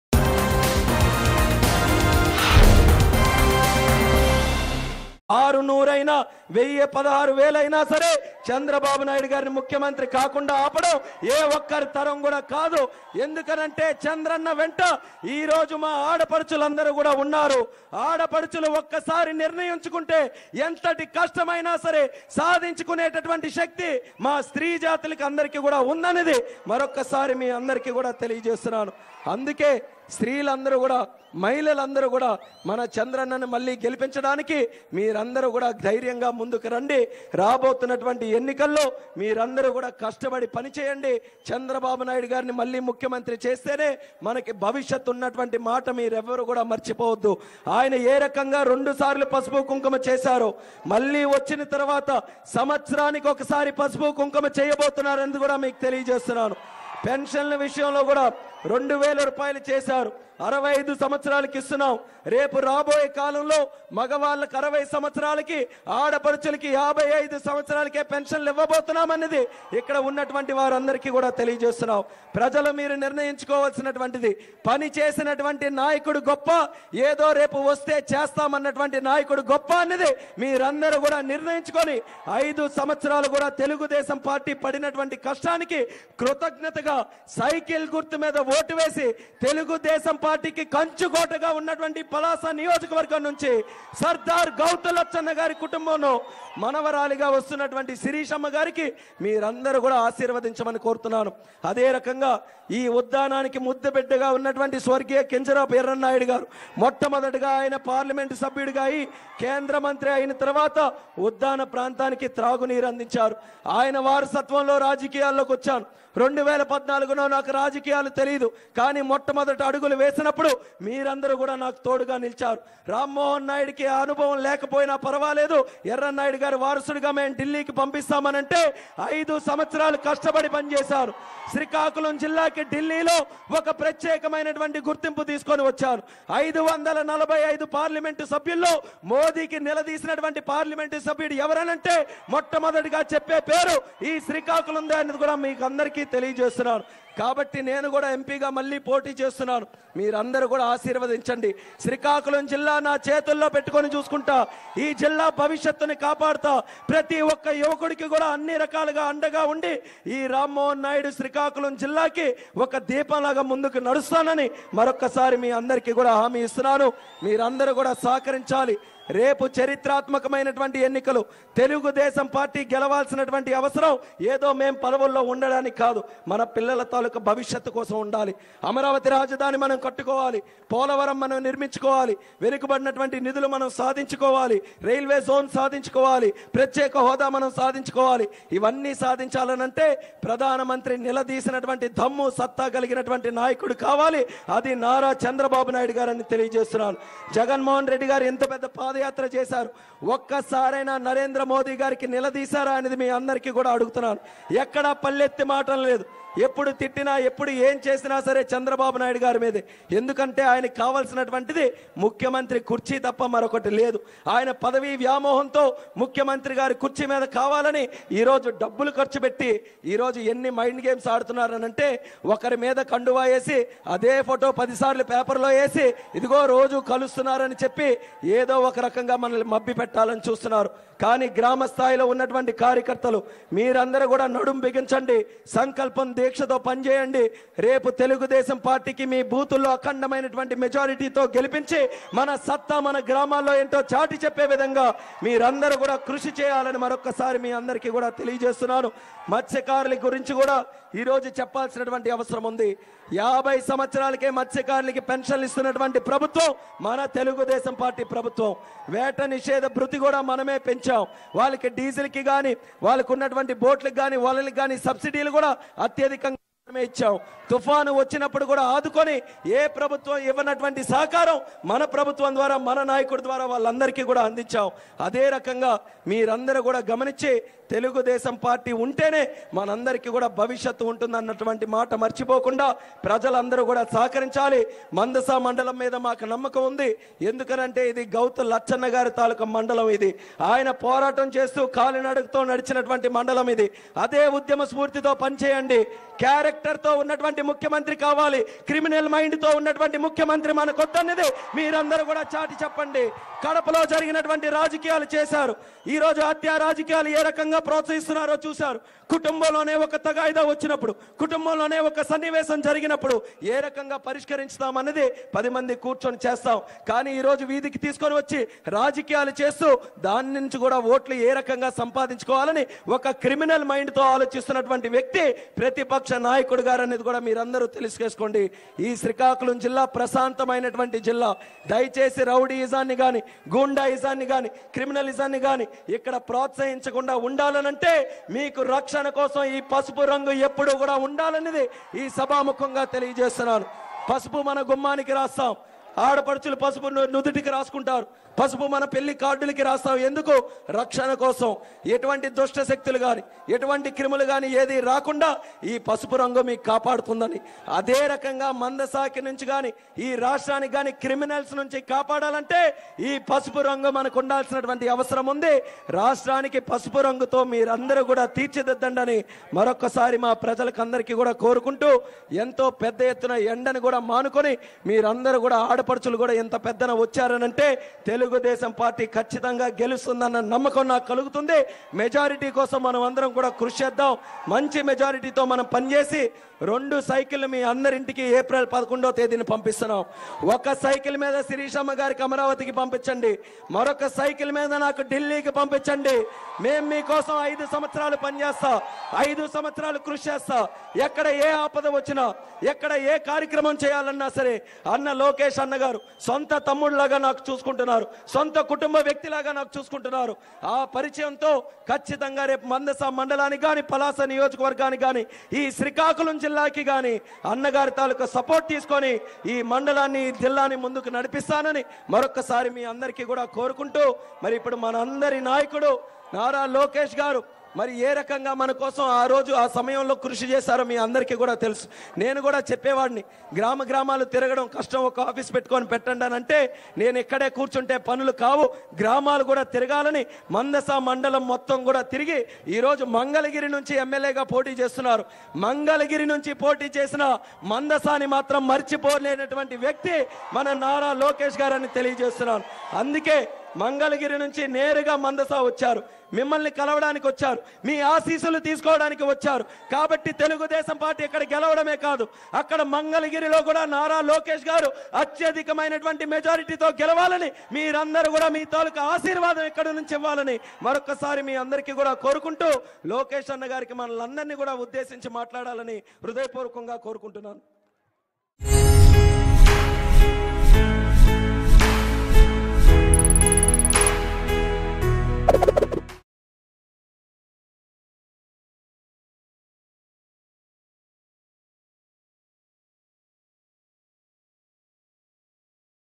Rammohan Naidu Praises Nara Lokesh | Rammohan Naidu Election Campaign | AP Elections 2019 |MangoNews
Rammohan Naidu Praises Nara Lokesh. He said that the development of the state will be possible with only Chandrababu Naidu and said that the pasupu kumkuma scheme will be given once in a years after winning in the elections.